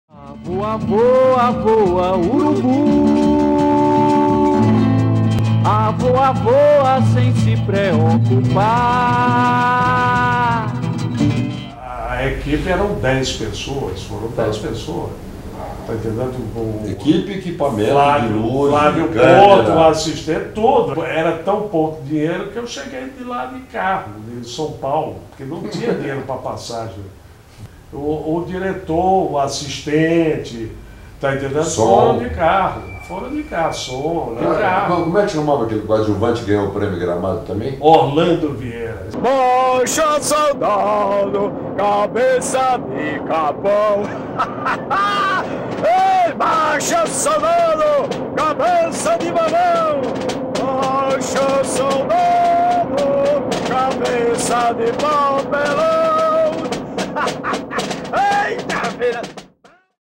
Blimp Film - Sargento Getúlio, Hermano Penna
Sargento Getúlio, baseado no livro homônimo de João Ubaldo Ribeiro, foi filmado em 16 mm no ano de 1978 e lançado depois de seu blow up para 35 mm  em 1983. O filme ganhou  6 prêmios no festival de Gramado e teve uma carreira aclamada no mercado internacional.

Ao lado de Guga de Oliveira, Hermano Penna, um dos grandes diretores de cinema que trabalhou na Blimp Film, conta-nos sobre as dificuldades de  produção desse grande sucesso.